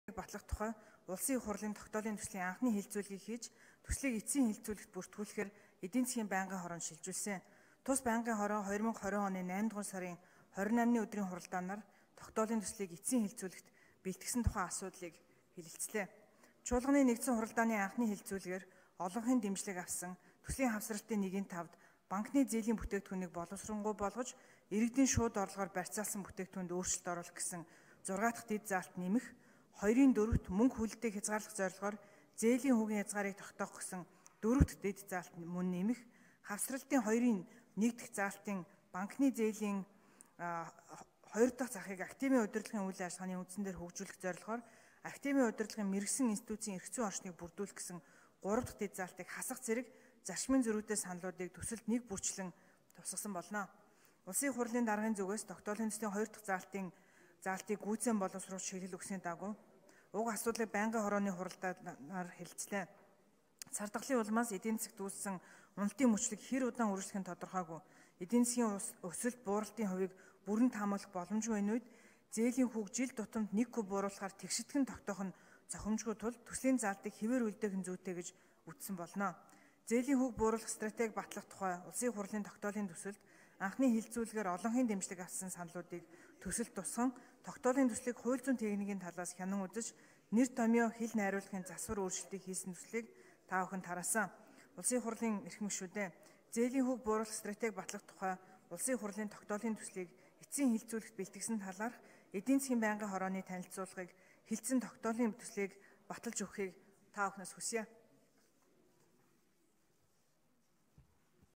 УИХ-ын гишүүн Х.Булгантуяа Улсын Их Хурлын тогтоолын төслийг танилцууллаа
УИХ-ын гишүүн Х.Булгантуяа “Зээлийн хүүг бууруулах стратеги батлах тухай” Улсын Их Хурлын тогтоолын төслийг танилцууллаа